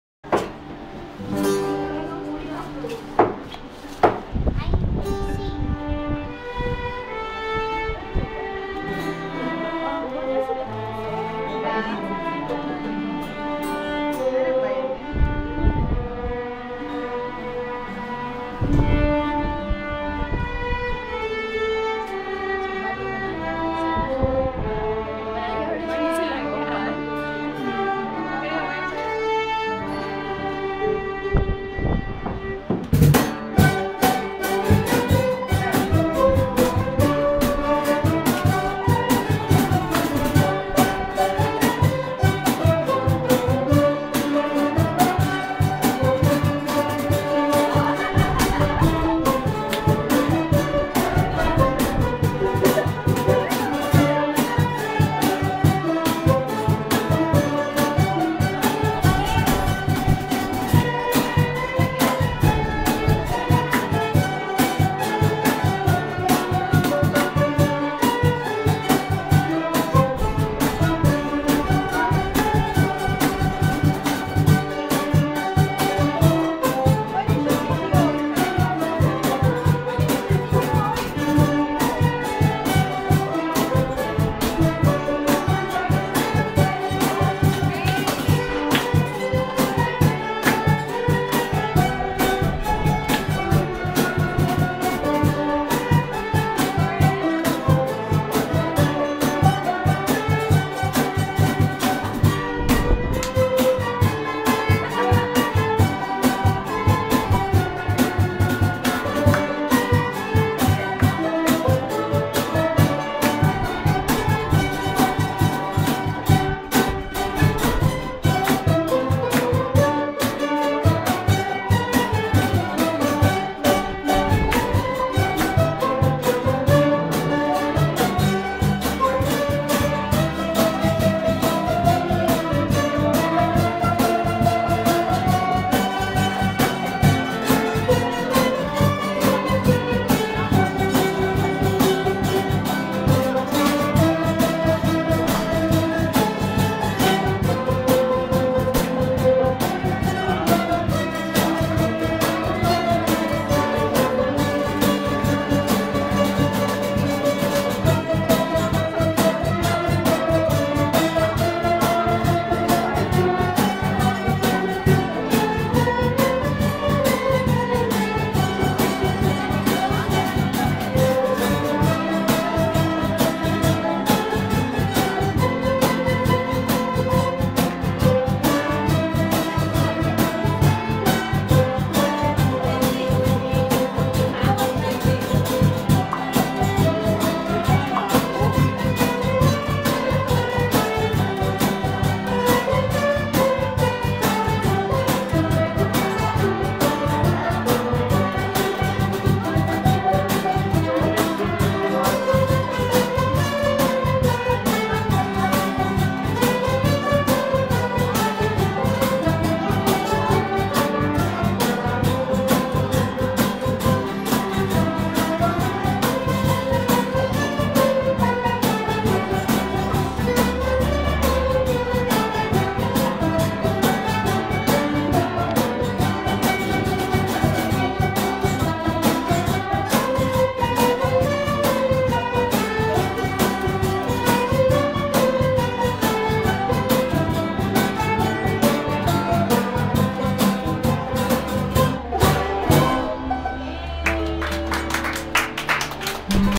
Children's Joy Foundation - Cebu - Dance Number
eTelecare Recruiting Team visits the Children's Joy Foundation for the Christmas Card Competition.